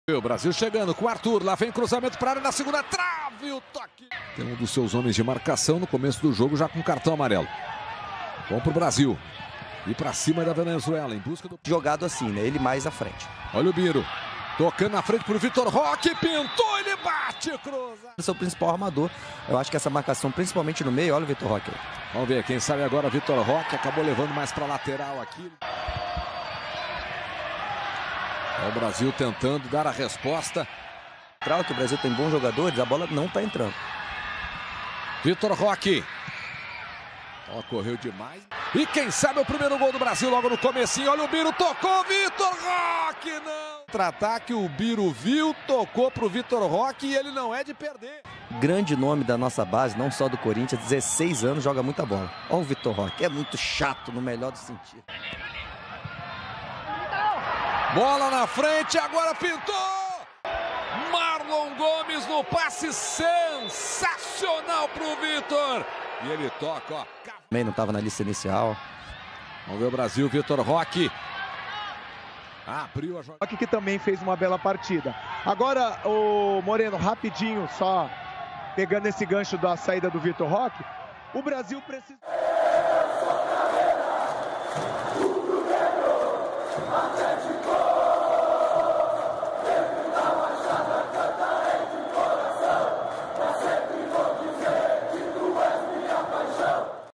VITOR ROQUE MARCOU O SEXTO GOL PELA SELEÇÃO BRASILEIRA SUB 20
Vitor Roque vs Venezuela | Sul-Americano 03/02/2023